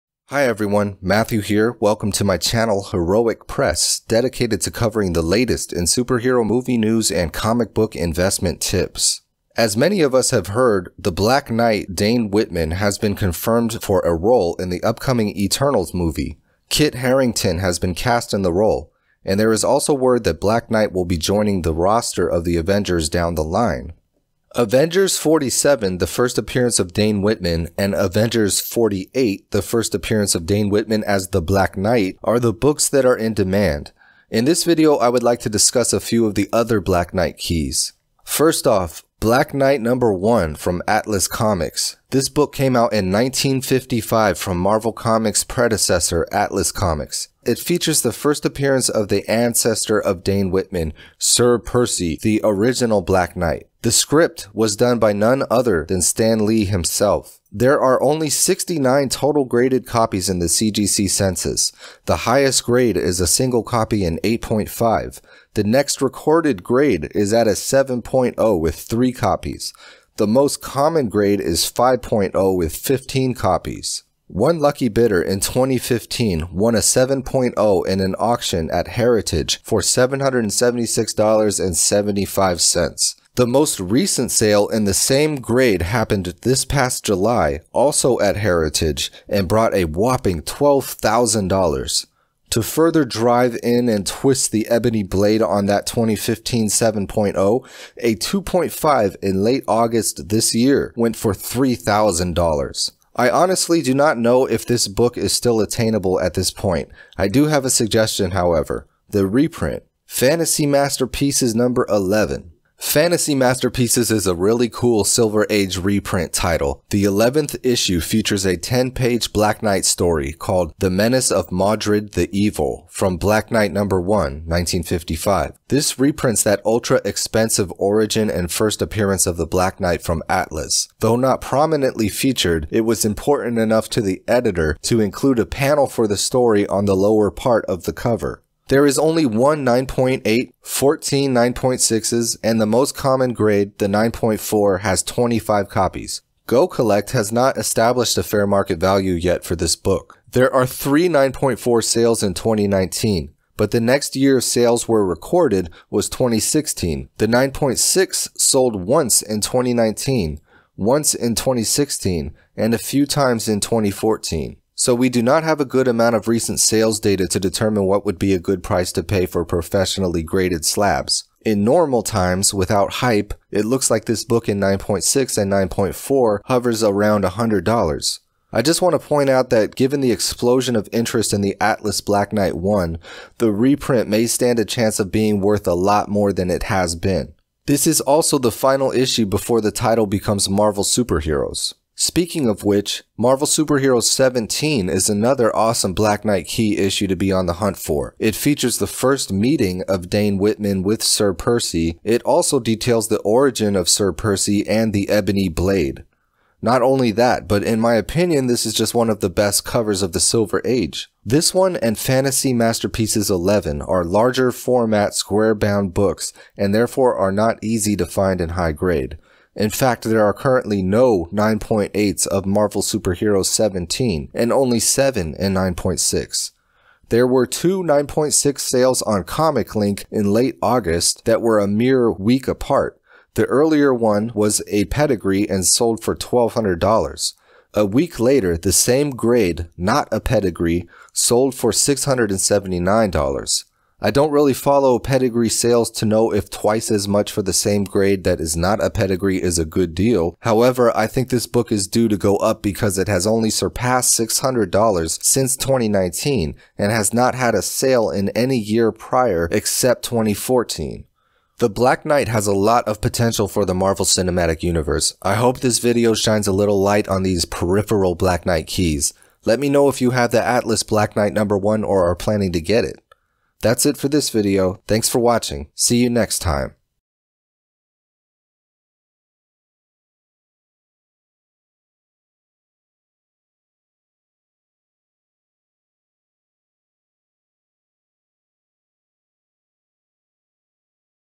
The Black Knight Comics To Get | Eternals | Marvel Phase 4
The Black Knight is being played by Kit Harrington in the upcoming Eternals movie. Here are some of the comics to be on the look out for.